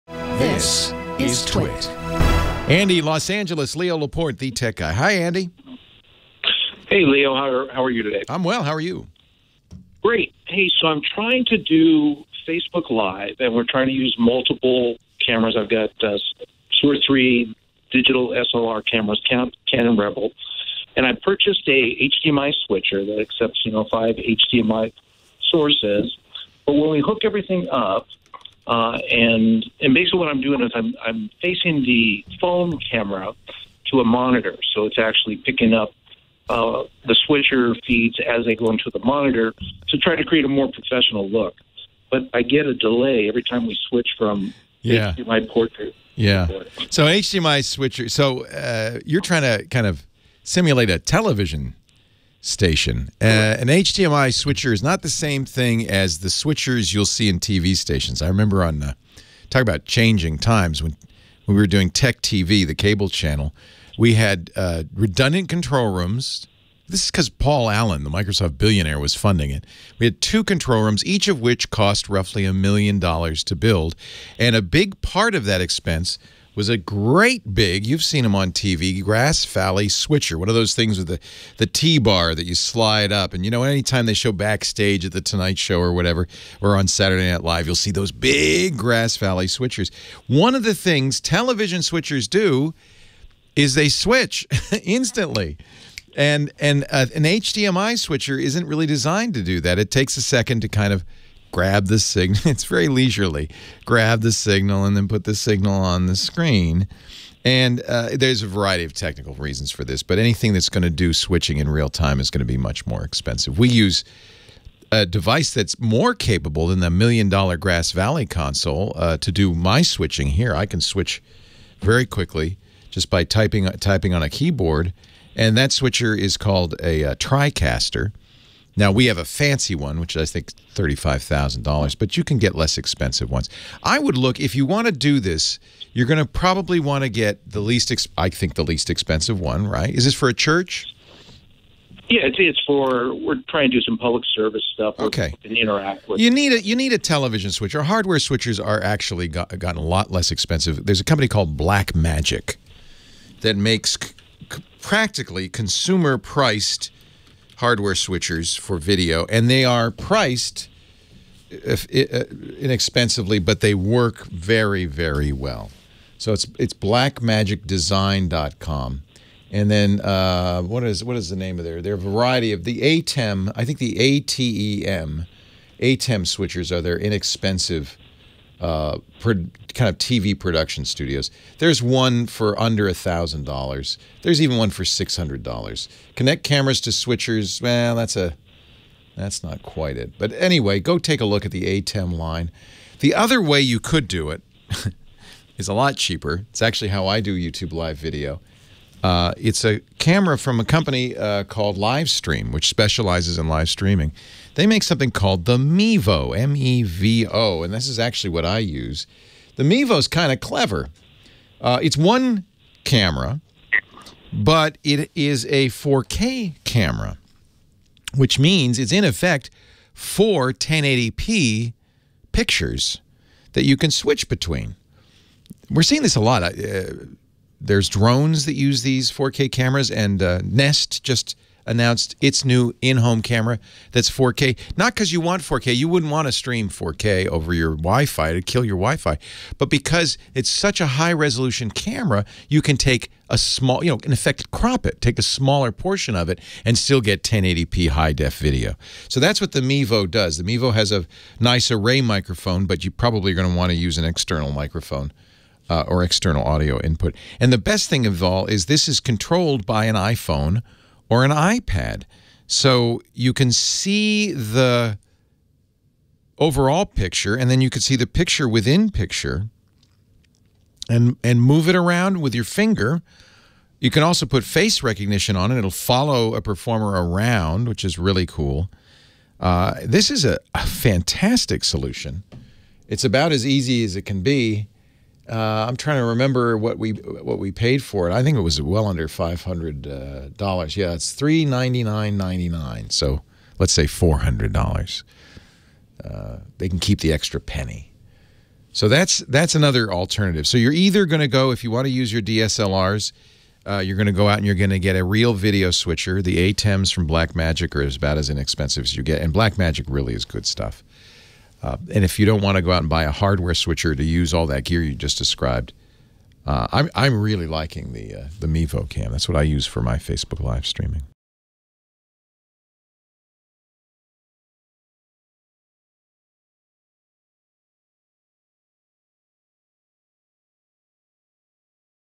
Stream to Facebook Live With Multiple Cameras
Leo Laporte explains how to switch between multiple cameras when streaming to Facebook Live.